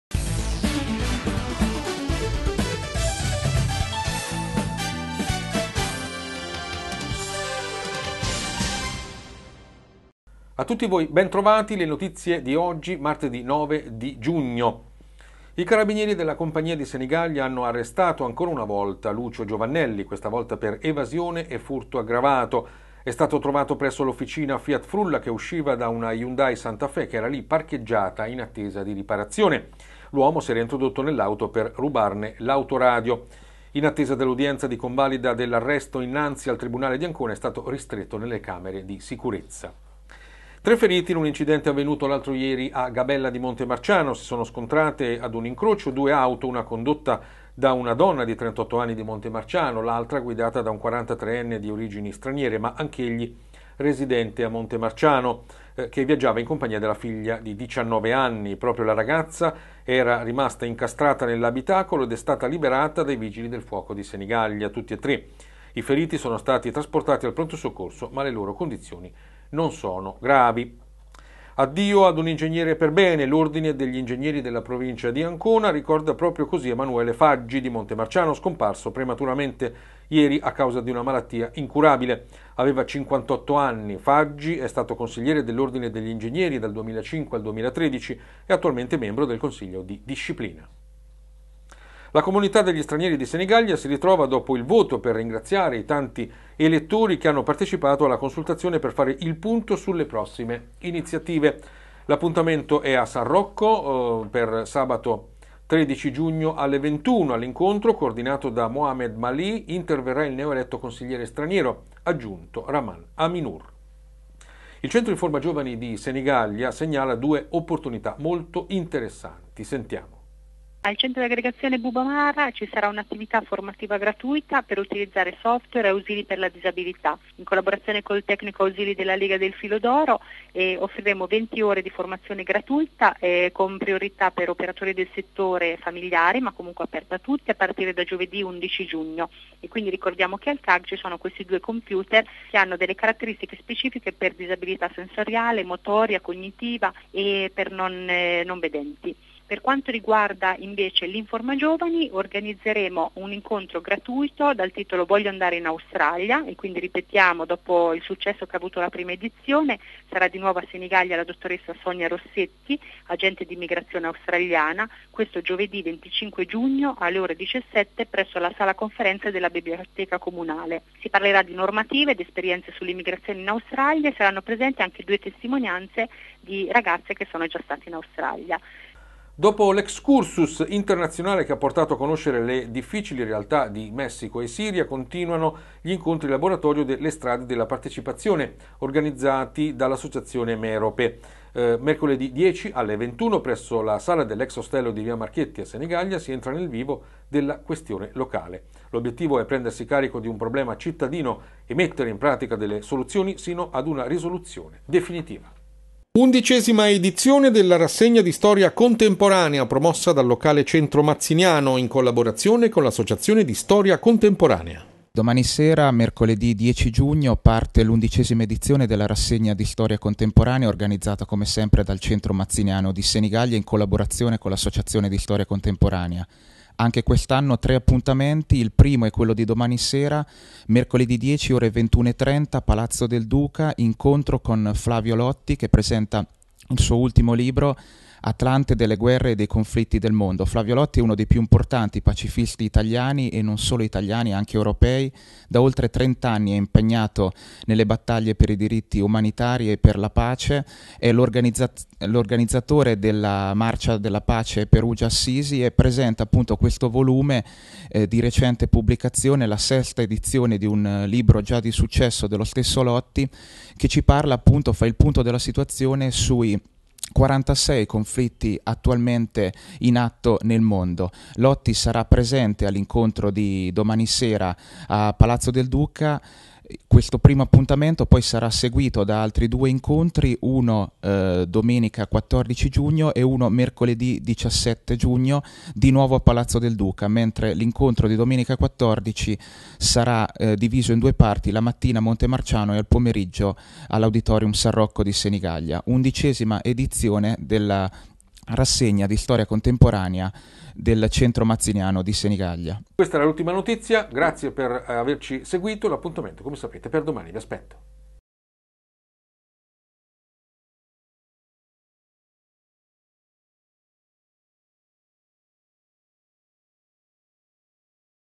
Notizie Senigallia WebTv del 09-06-15
Titoli del Videogiornale di oggi 09-06-15 di Velluto Senigallia RADIO & WEB TV: i Carabinieri arrestano di nuovo Lucio Giovannelli, tre feriti in un incidente d'auto alla Gabella di Montemarciano, le informazioni dell'informagiovani.